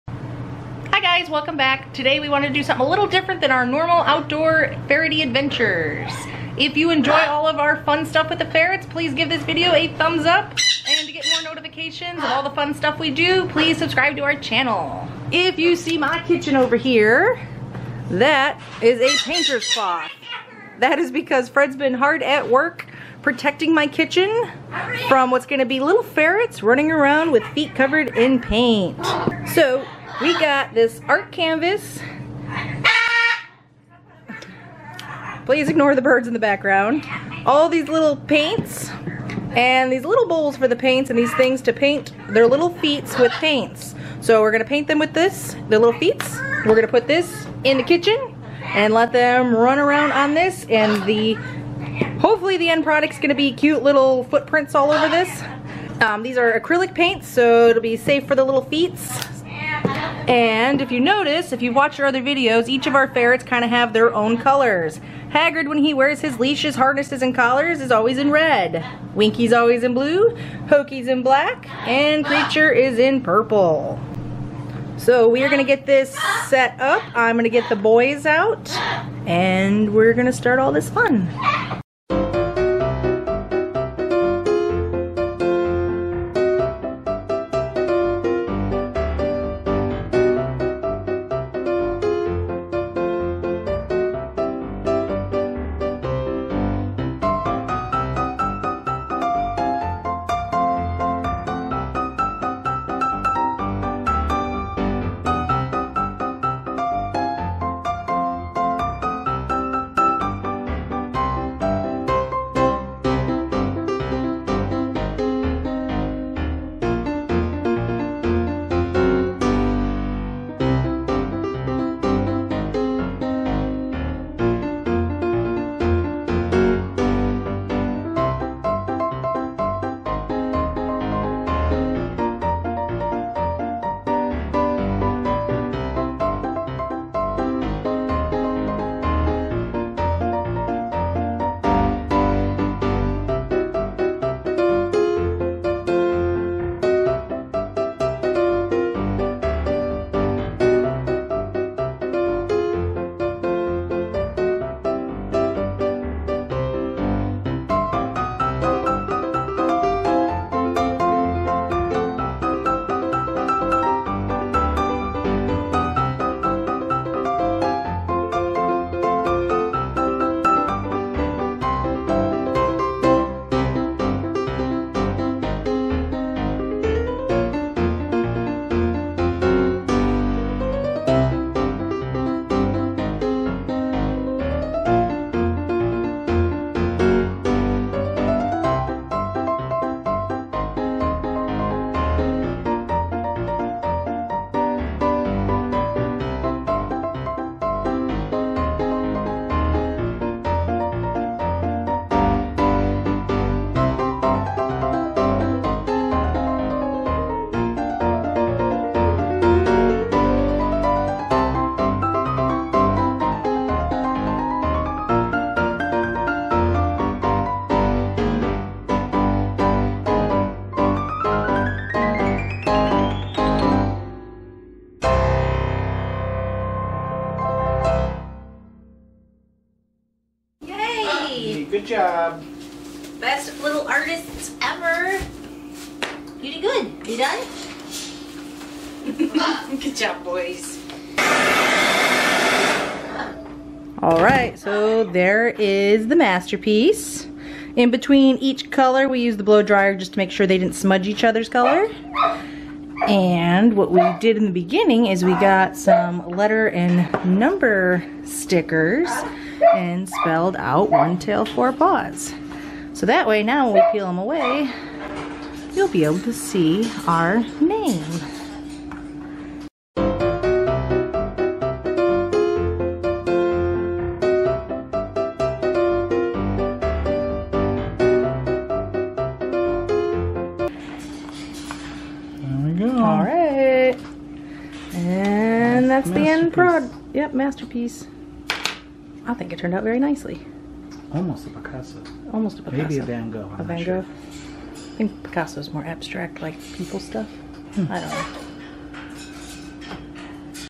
PAINTING WITH FERRETS | Ferret Art
Painting with your Ferret is a great way to not only have fun with your ferret by making ferret art, but to make a great item to remember your ferret by. You can actually do this with any of your pets.
Please remember to make sure to use non toxic paints (some acrylic paints are toxic). Even with non-toxic paints, it is important that you make sure your pet is not ingesting (eating) the paint, and that you give them a bath right after so they don't lick the paint off of themselves later.

Have fun!

*LiveLifewithYourPets*